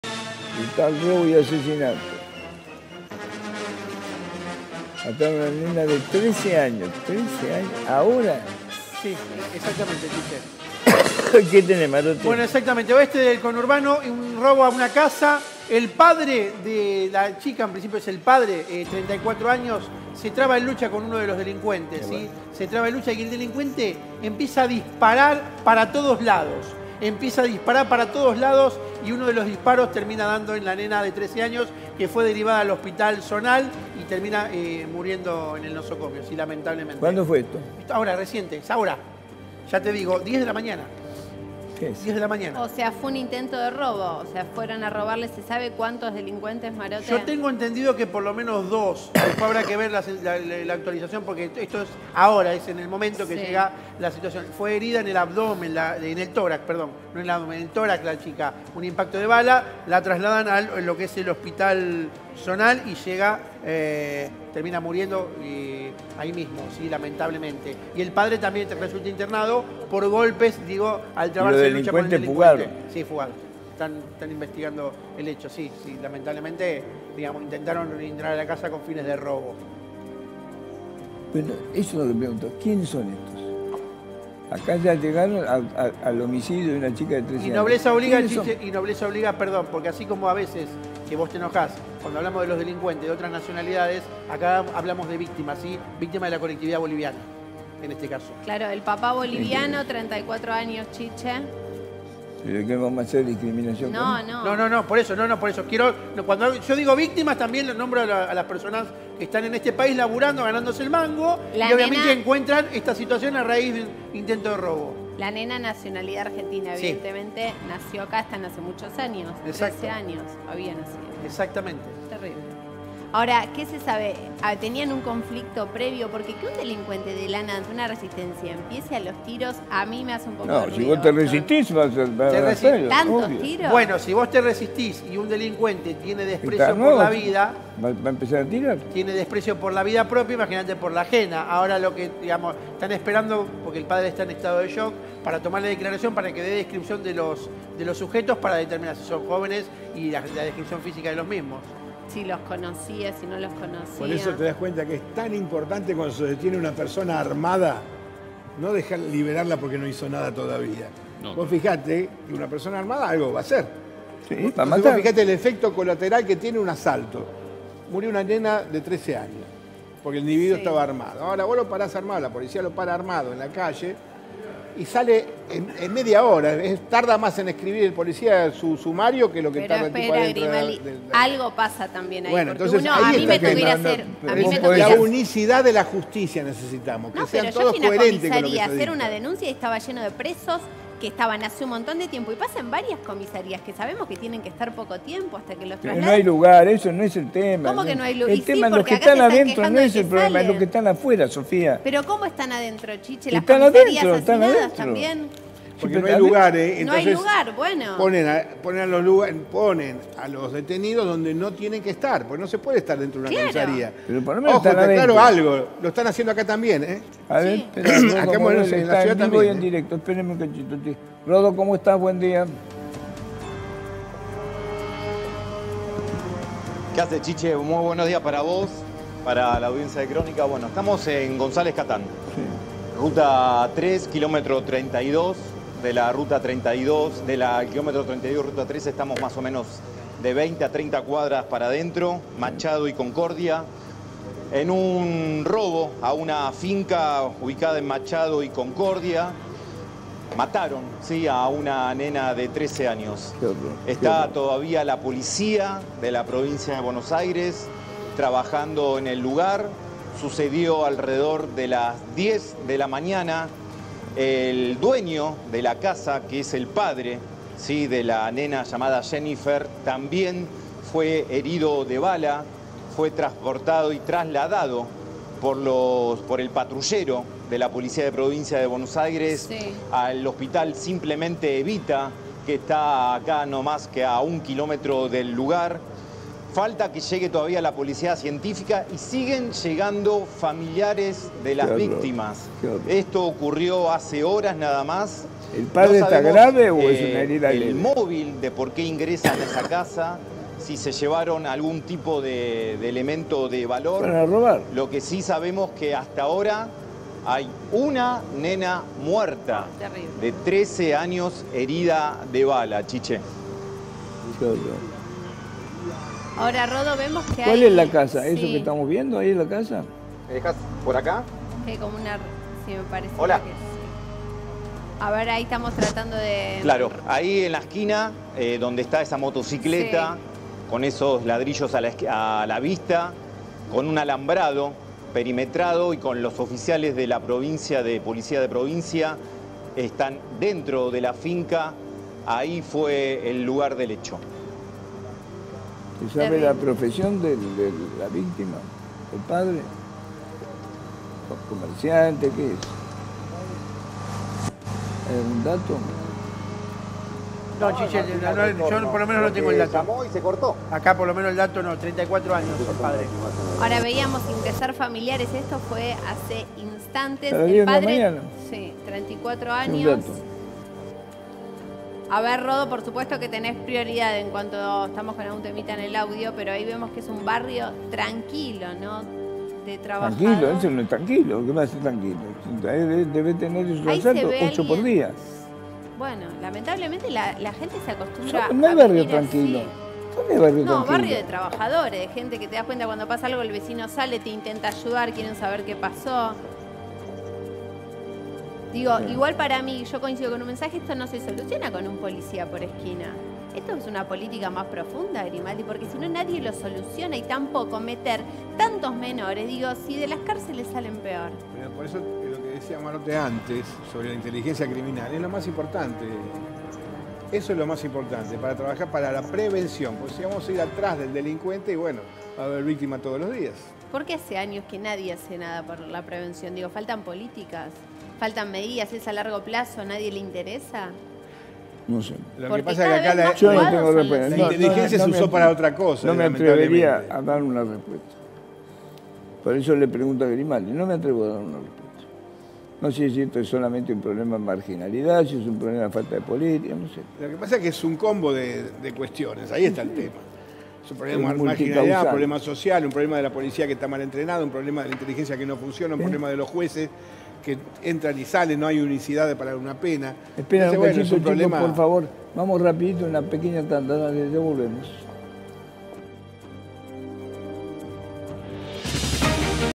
Brutal robo y asesinato: mataron a una menor de 13 años
Nena de 13 años se interpuso entre su papá y los delincuentes y fue asesinada de un disparo en el pecho

El crimen ocurrió esta mañana, cerca de las 10, cuando al menos dos delincuentes armados ingresaron al domicilio de la familia con intenciones de robo, de acuerdo a la primera hipótesis. Por el momento no hay detenidos.